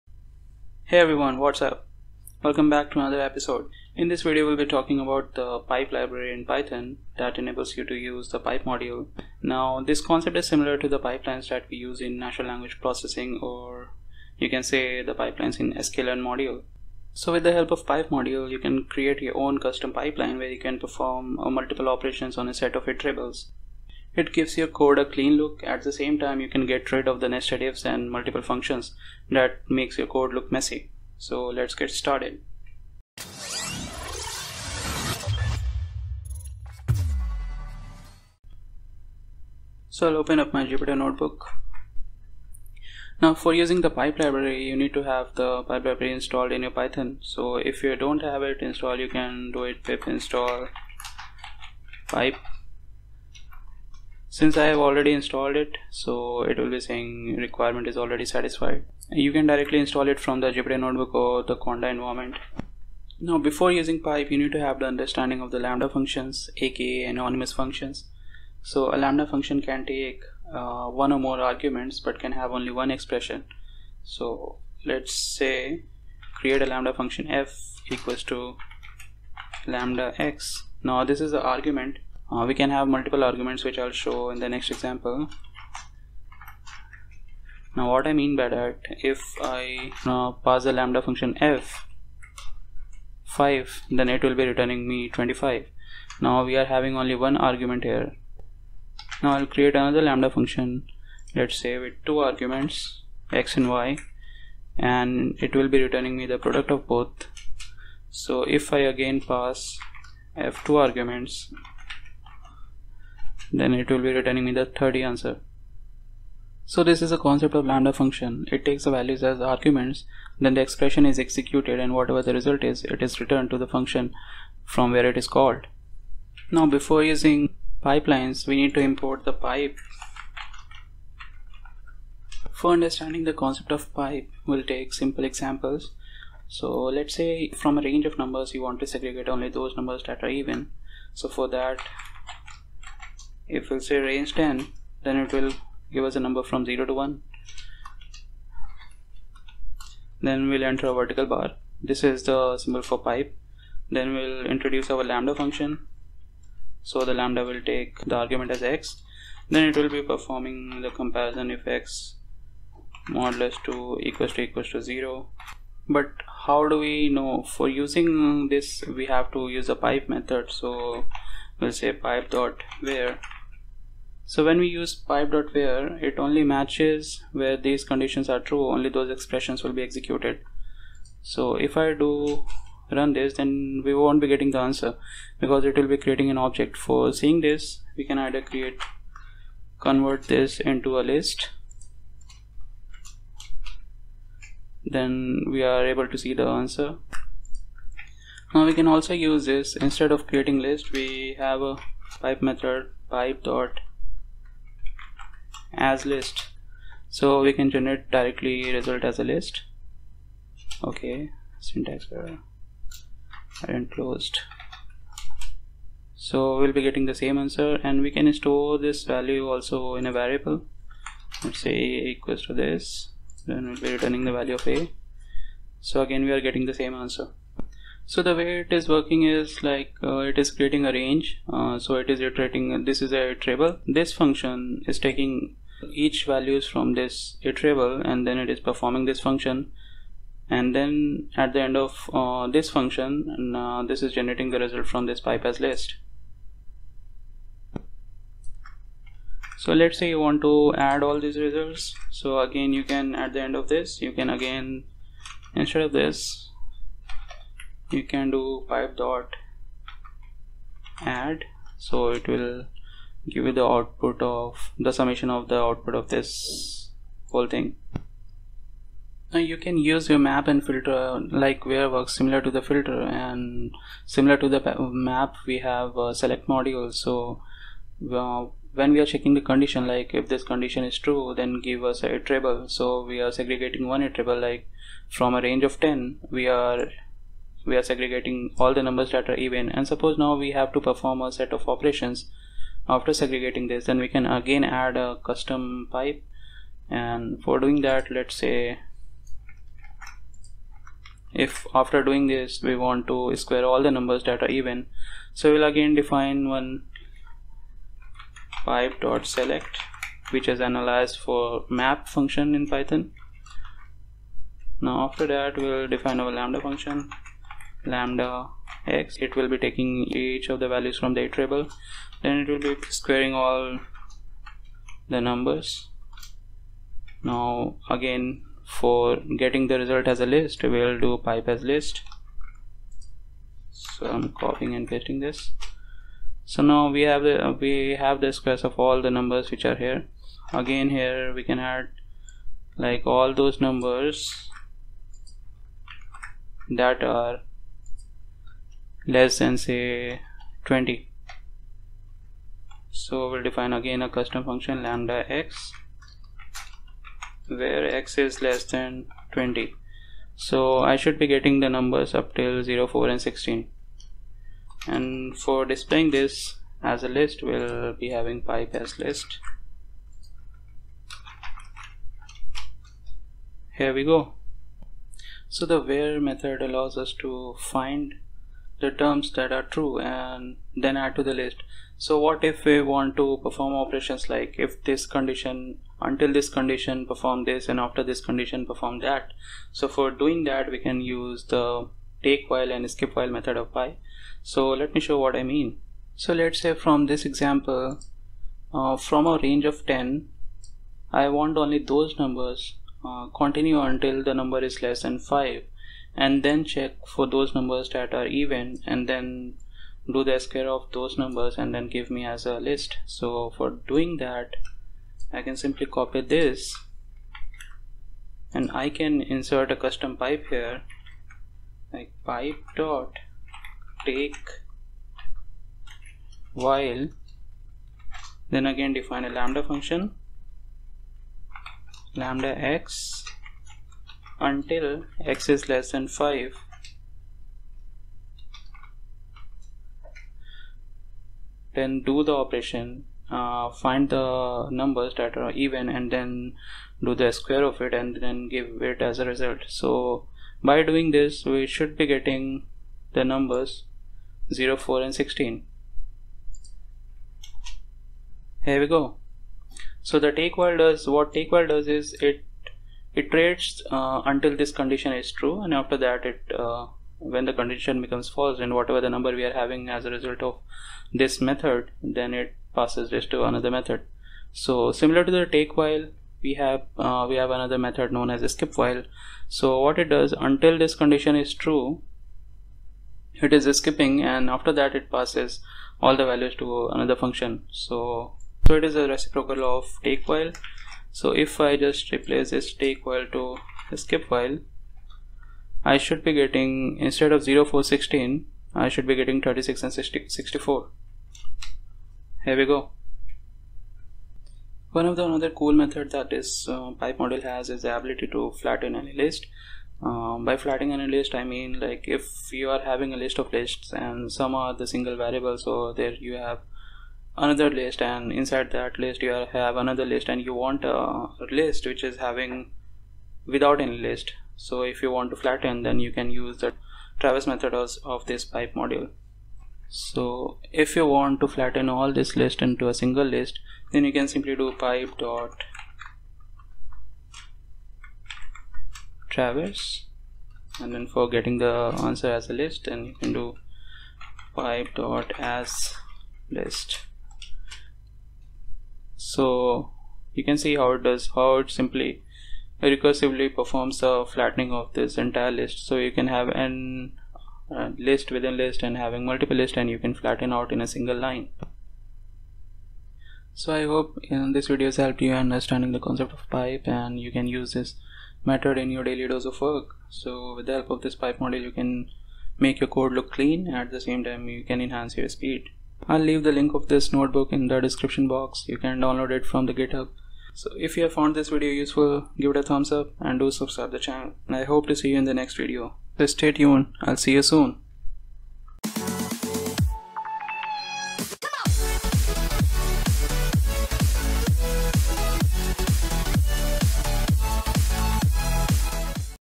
How to create pipelines in python | Pipe | Python Tutorial
Hey everyone and welcome back to another video of MechBuzZ !!!

In this video, we'll try to understand the concepts of pipe in python.

Pipe is a Python library that enables you to use pipes in Python.

A pipe (|) passes the results of one method to another method.

You can create your custom pipeline for a number of operations in neat and clean way.

NEW VIDEO/S EVERY WEEK!

How to make pipelines in python
pipe library in python
python pipeline